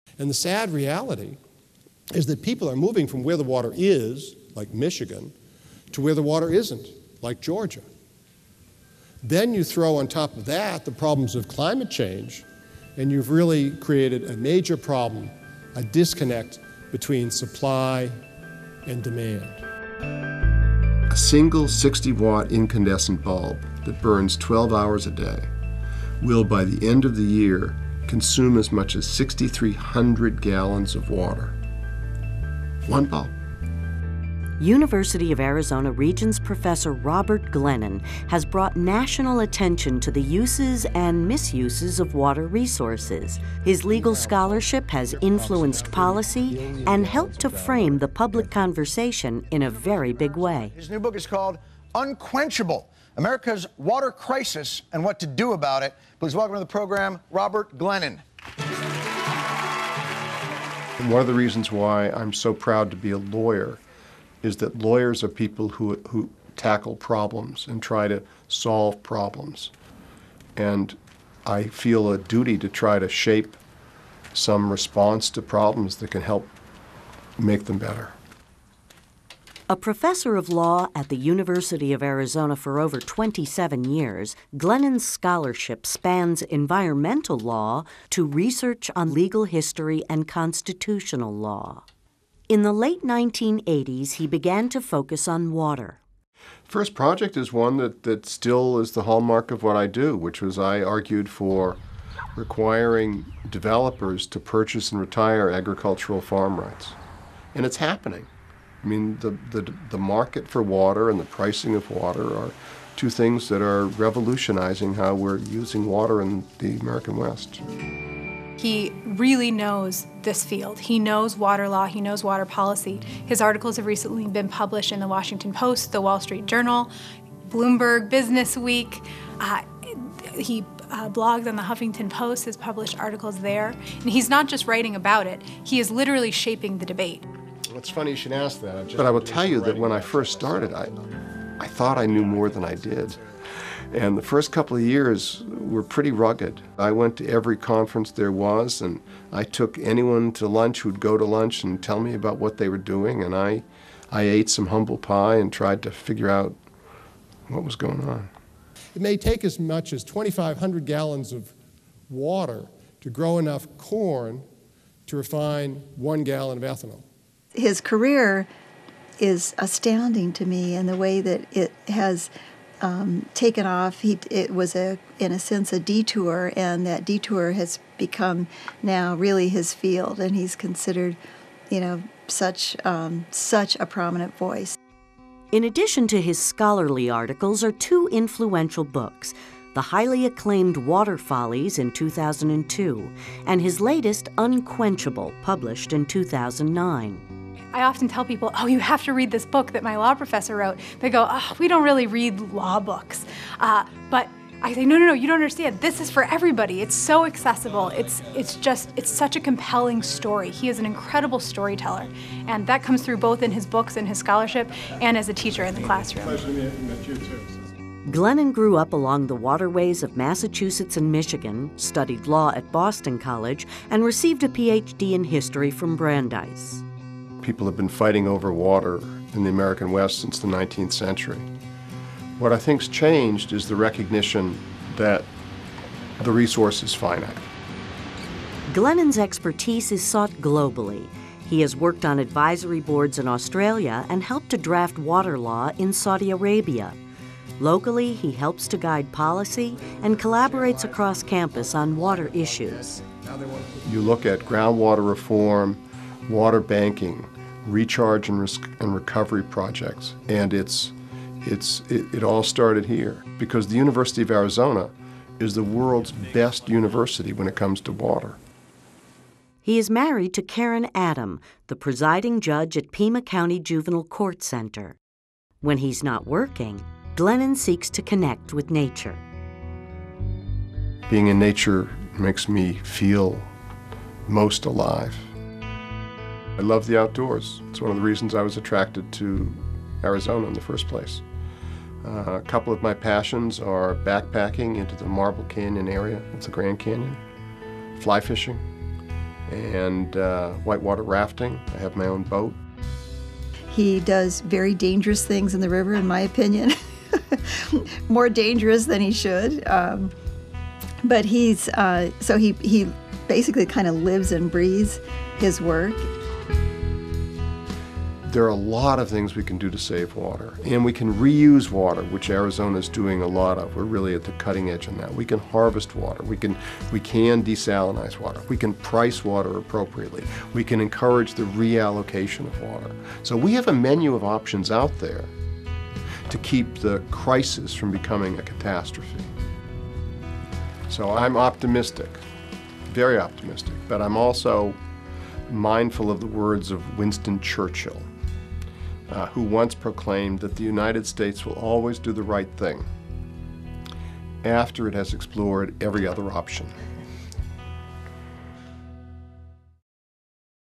Prof. Robert Glennon - In Defense of Water
Water resource expert Robert Glennon shares why America's water crisis drives him to fight for nationwide changes in environmental policy.